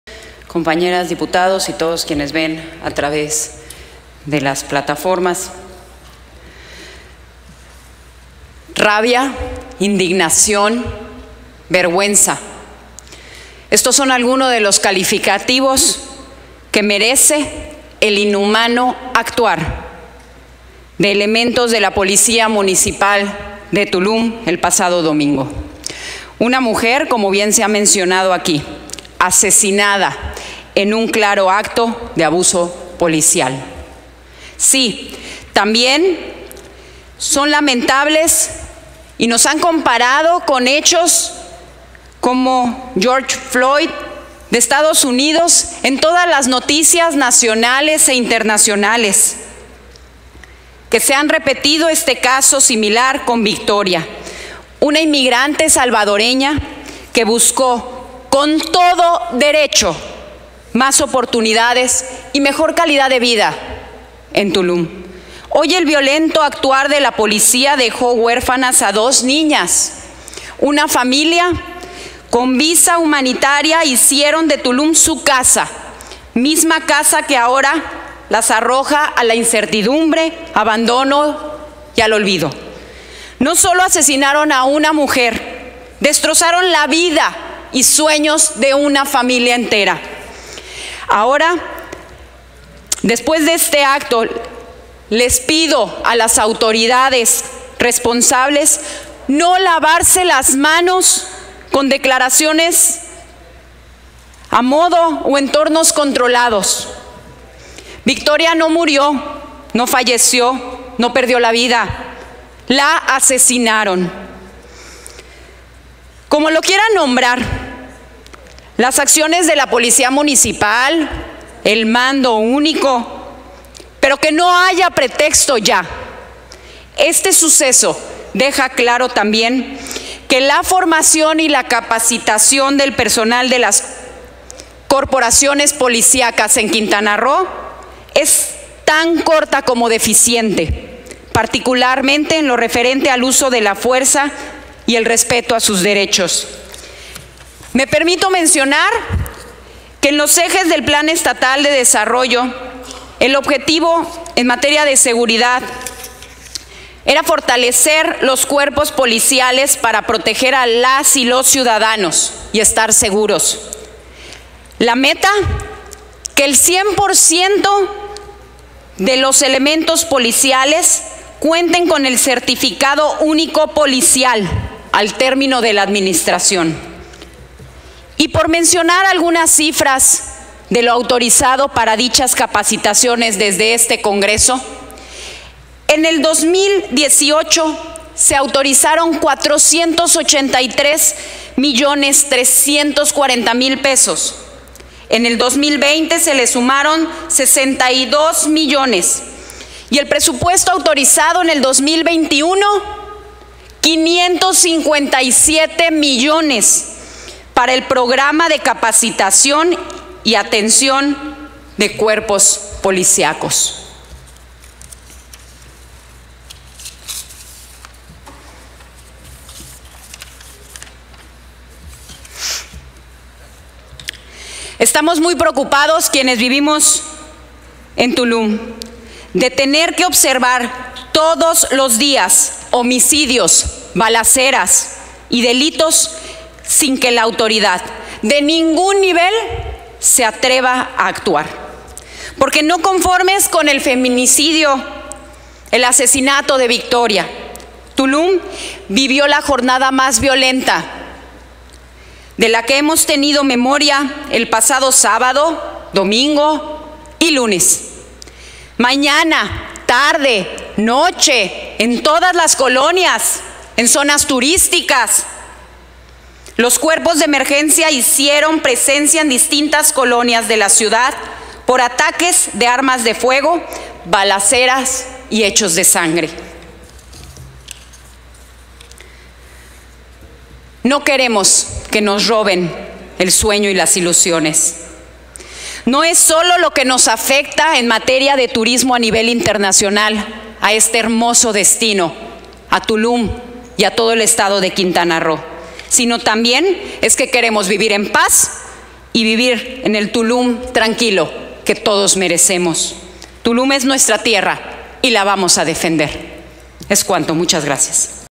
Intervención de la Dip. Euterpe Alicia Gutiérrez Valasis. MORENA (30/Mar/2021)
Intervención de la Diputada Euterpe Alicia Gutiérrez Valasis, de MORENA, en relación a los acontecimientos de violencia en el Estado.